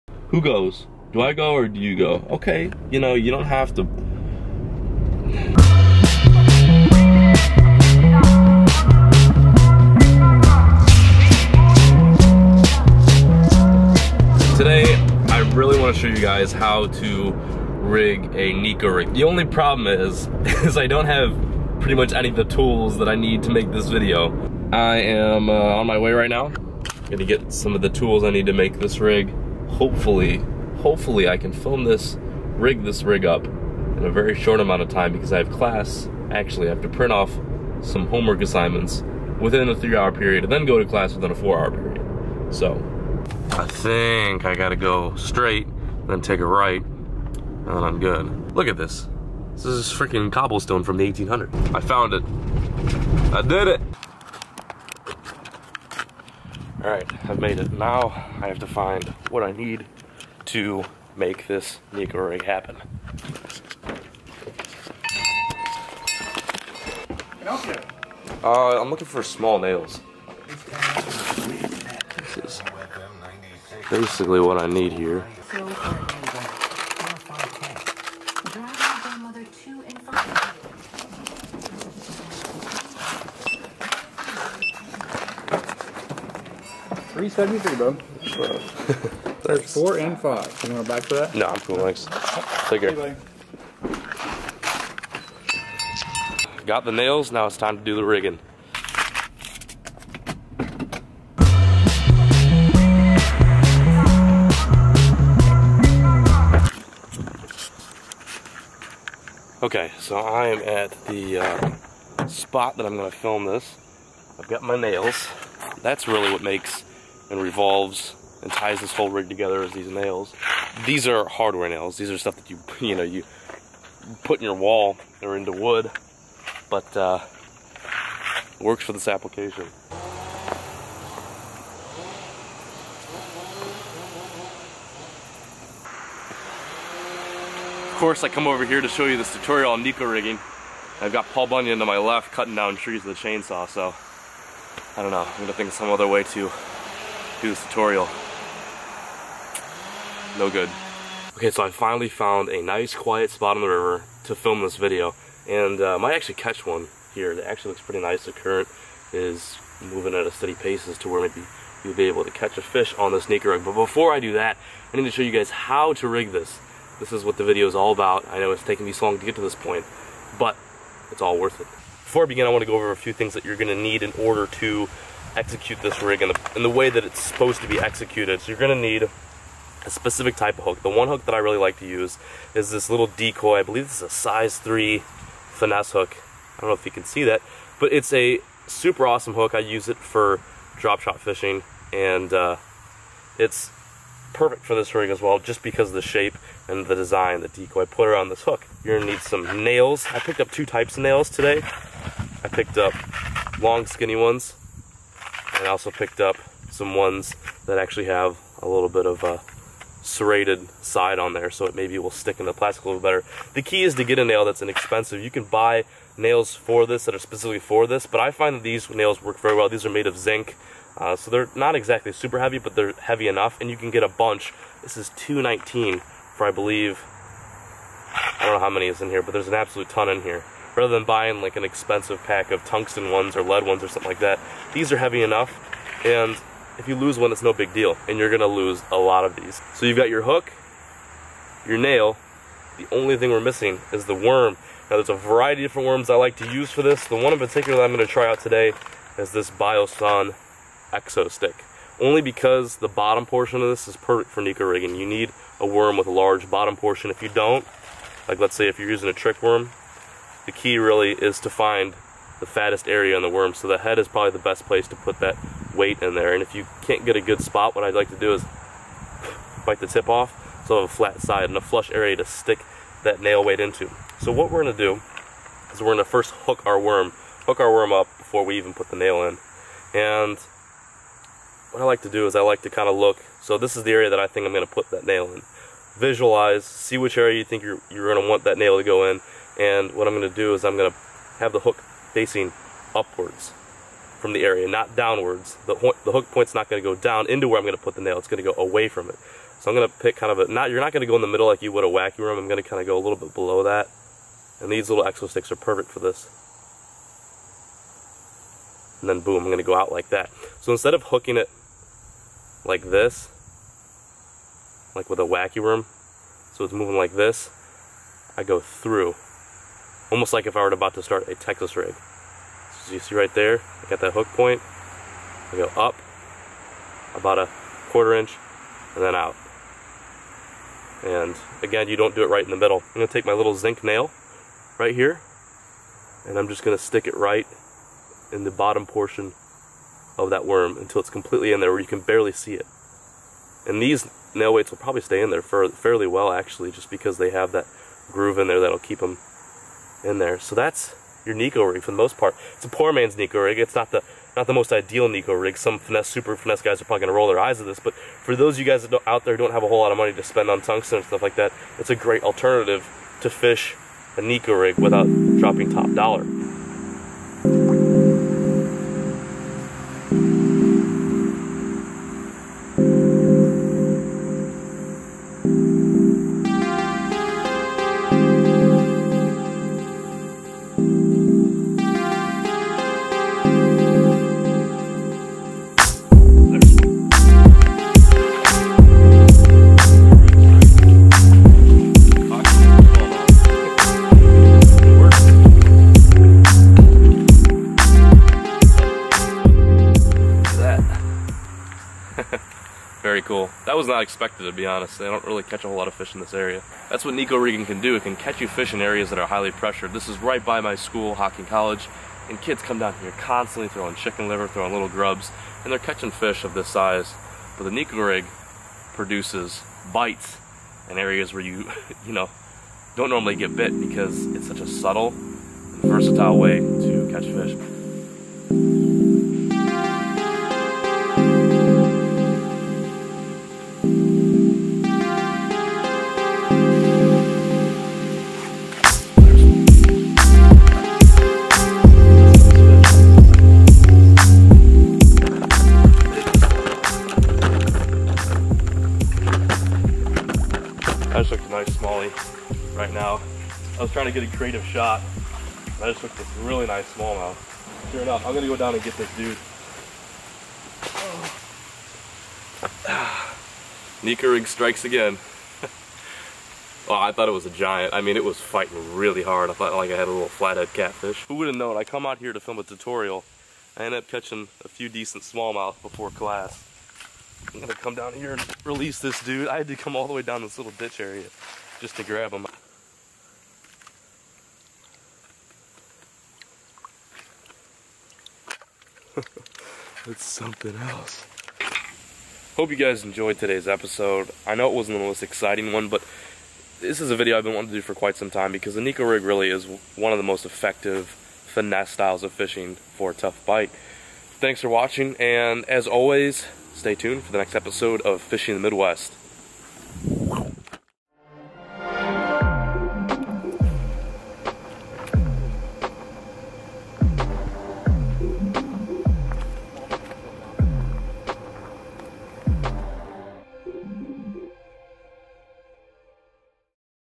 Neko Rig -- How to Rig It and Fish It + River Smallmouth
The Neko Rig isn’t just for top dog finesse anglers, it can be an effective and fantastic technique for young guns looking to fishponds, small lakes, and even rivers. The idea of throwing such a strange rig maybe hard to commit to but as long as you put in the hours you will become a better Neko Rig angler than you were the day before.

Wait, so you like that sweatshirt? Here, buy one